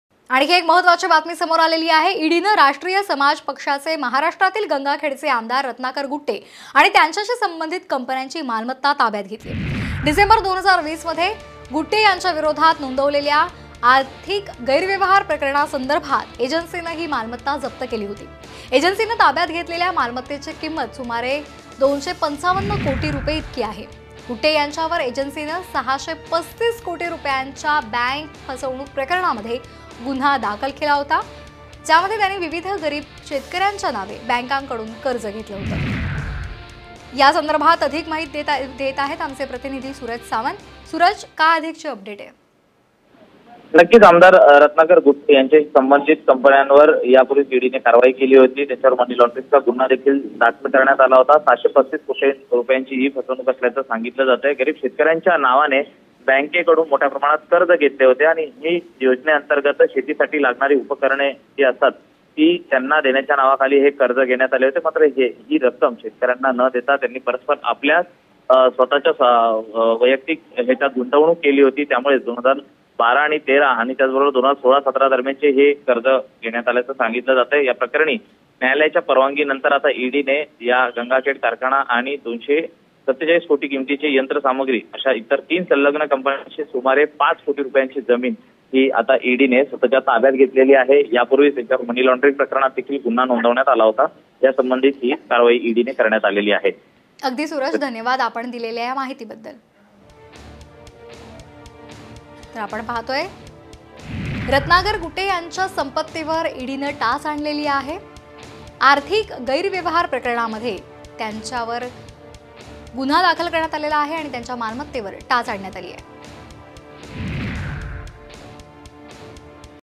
MLA Ratnakar Gutte: आमदार रत्नाकर गुट्टेंना ईडीचा दणका | Politics | Maharashtra | Sarkarnama
मुंबई: अंमलबजावणी संचालनालयाने (ईडी) राष्ट्रीय समाज पक्षाचे (आरएसपी) महाराष्ट्रातील गंगाखेडचे आमदार रत्नाकर गुट्टे (Ratnakar Gutte, MLA) आणि त्यांच्याशी संबंधित कंपन्यांची जप्त केलेली मालमत्ता ताब्यात घेतली आहे. (MLA Ratnakar Gutte hit by ED)